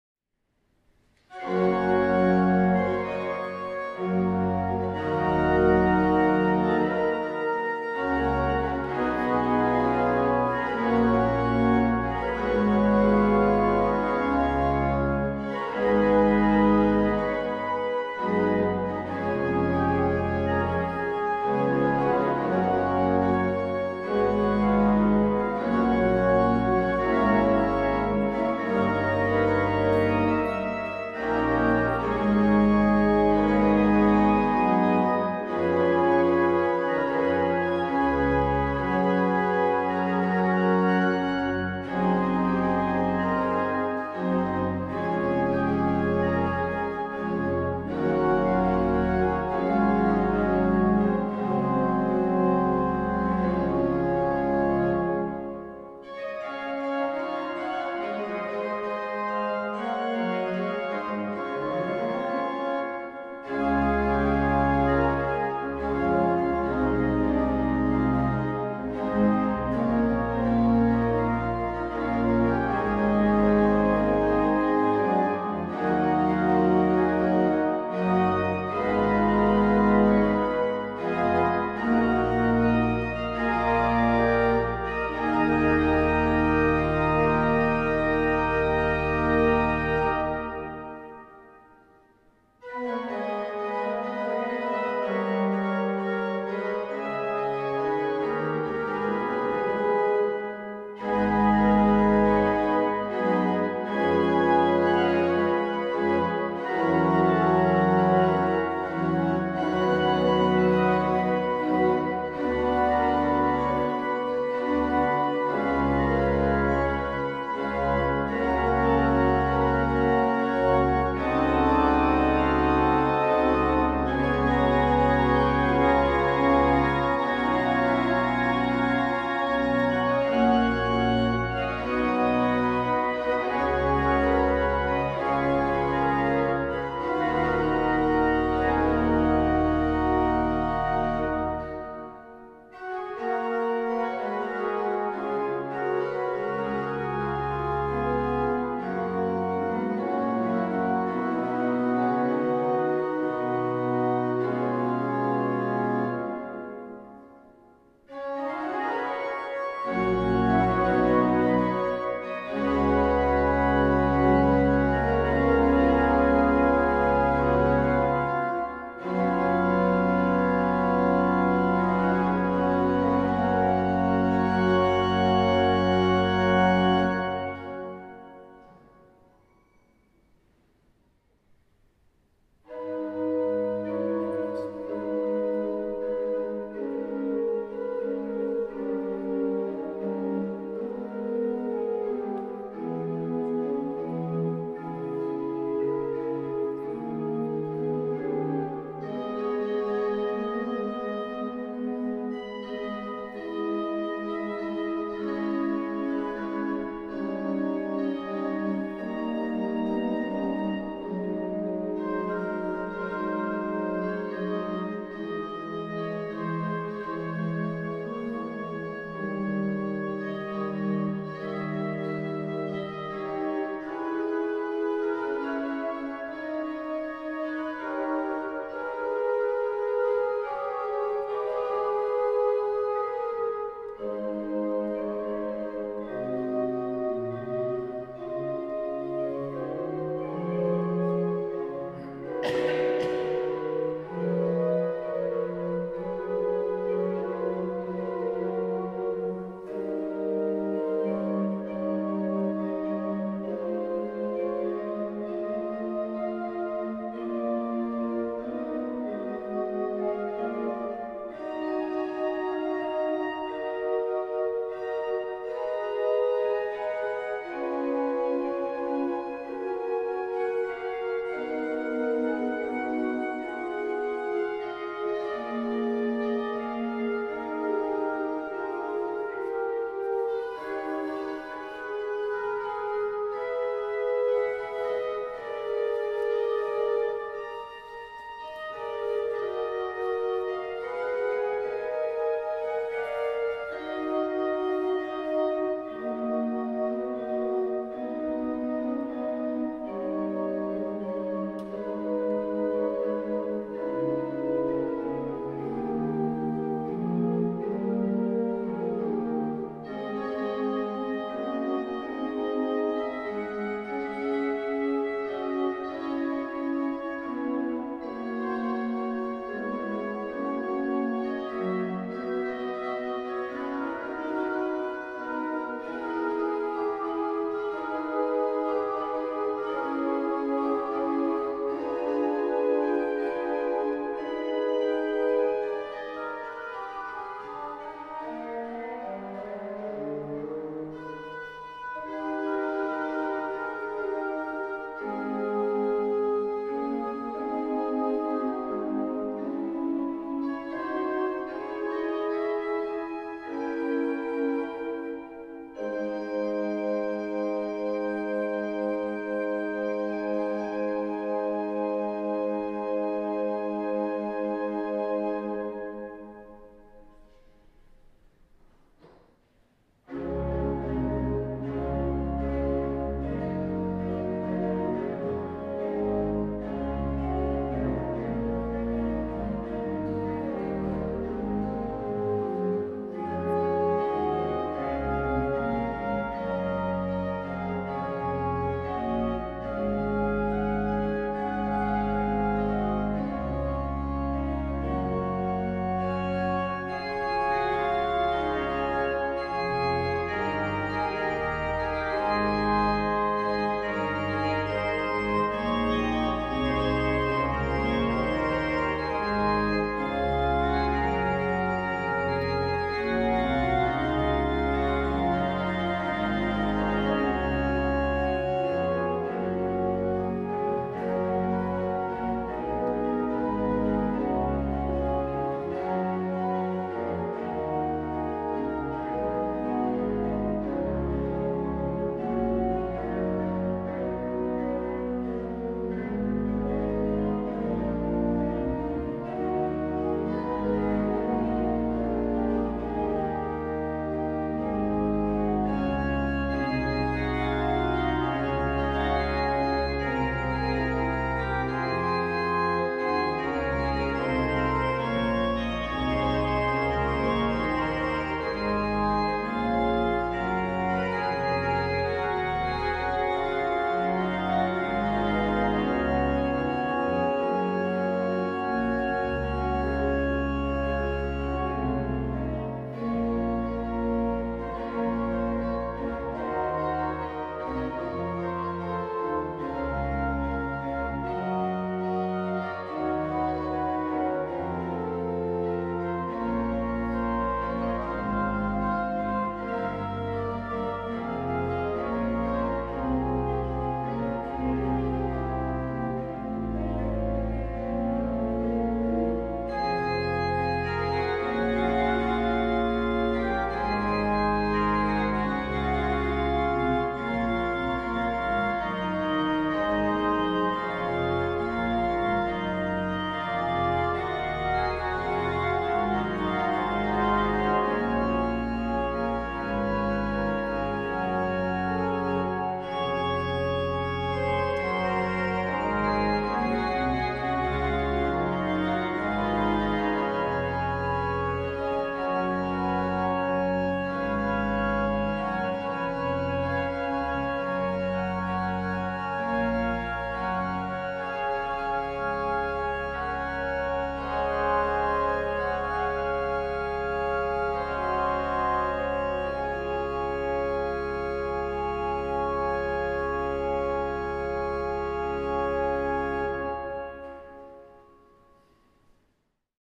Utazás a Holdba - barokk improvizációk a Hold utcai református templom orgonáján
A türingiai stílusú orgonát építette az AerisOrgona Kft.

Az élő koncertfelvételek időpontjai:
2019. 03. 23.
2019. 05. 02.